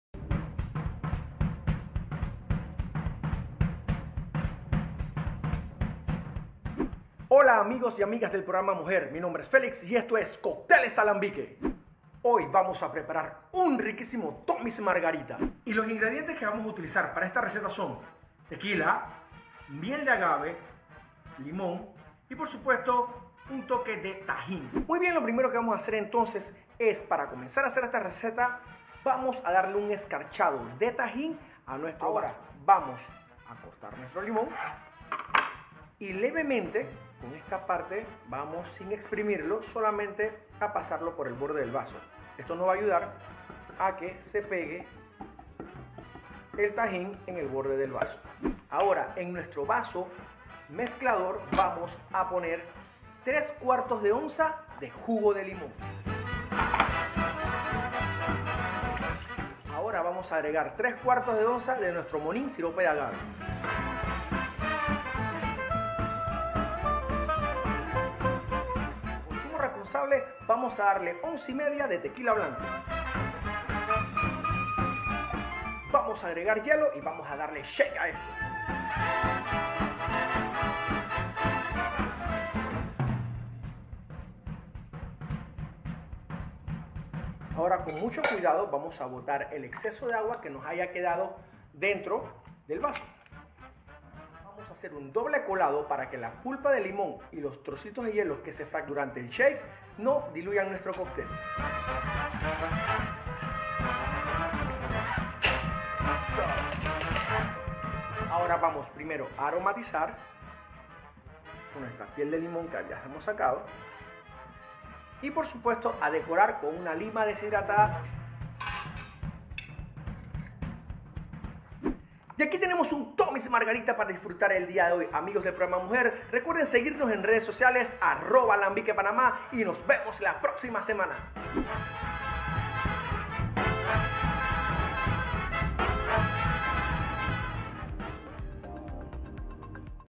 Tommy's Margarita
HISTORIA
El Tommy's Margarita fue elaborado entre 1987 y 1988 por Julio Bermejo, llamado así por el restaurante mexicanos  de sus padres en San Francisco.
Este cóctel aparece hoy día en los menús de los bares de todo el mundo, convirtiéndolo en un clásico moderno.
El Tommy's  Margarita a  diferencia de la Margarita tradicional, no lleva triple sec y sustituye el sirope simple por sirope de agave, para resaltar las notas del tequila.